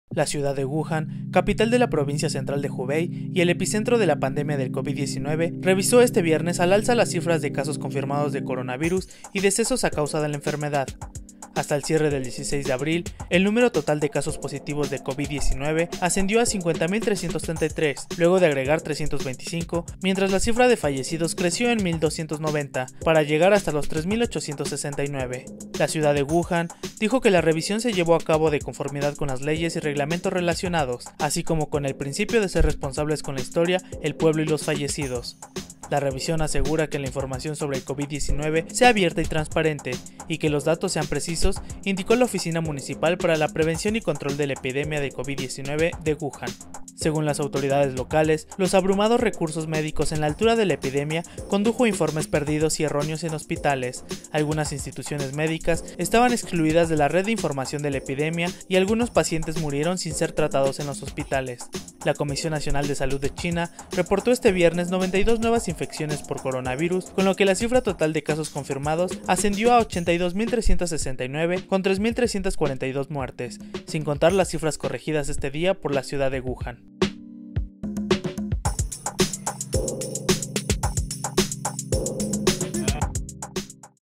Wuhan revisa al alza la cifra de muertes y contagios por coronavirus
La ciudad de Wuhan, capital de la provincia central china de Hubei, y epicentro de la pandemia de COVID-19, revisó este viernes al alza las cifras de casos confirmados de coronavirus y decesos a causa de la enfermedad.